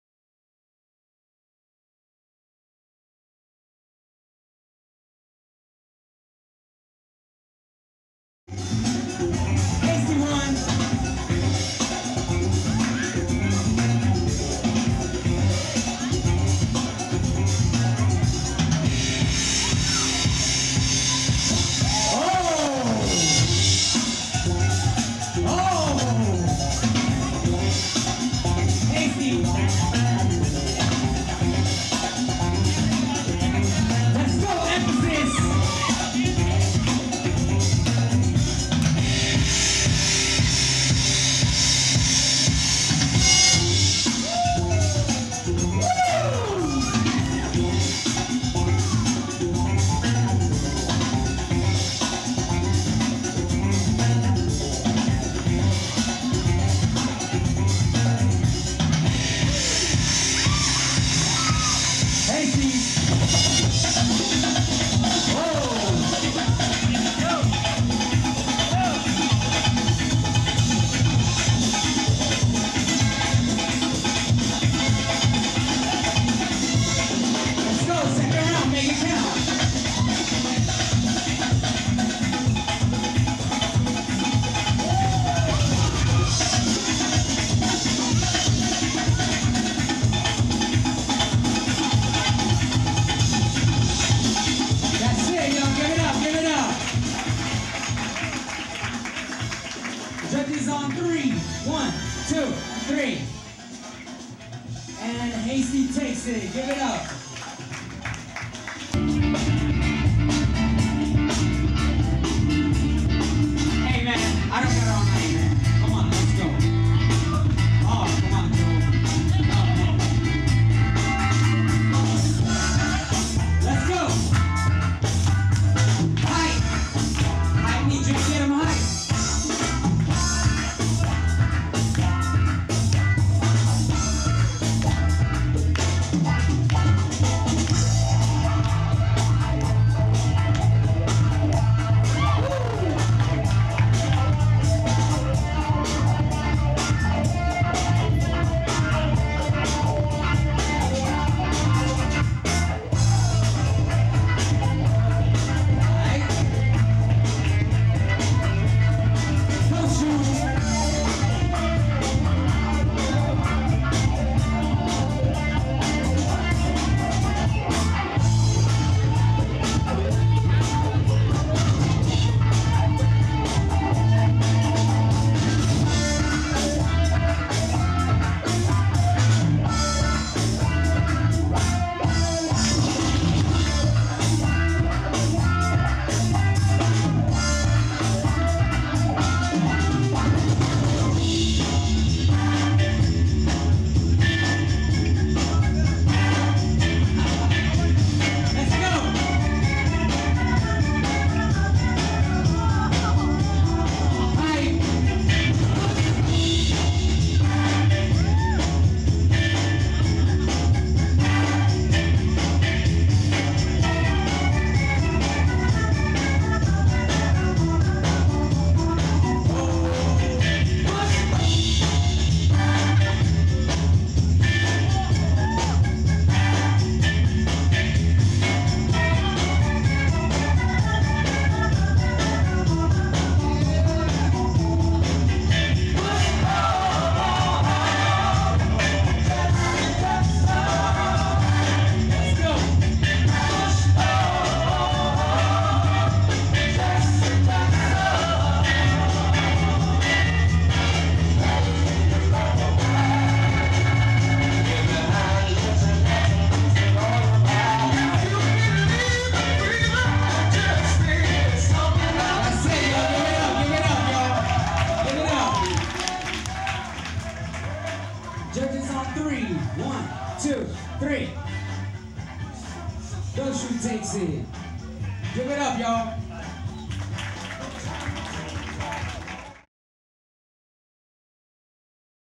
Catalyst bBoy Battle June2011 Semi 03.mov
B-Boy Battles at Catalyst Miami Springs, Florida.
Vicious, Kid Zeku, Hasty, Spindian, El Danny, Void, Kid Hype, Method, Lil Rocket, Statik, and more B-Boys battling for the top spot.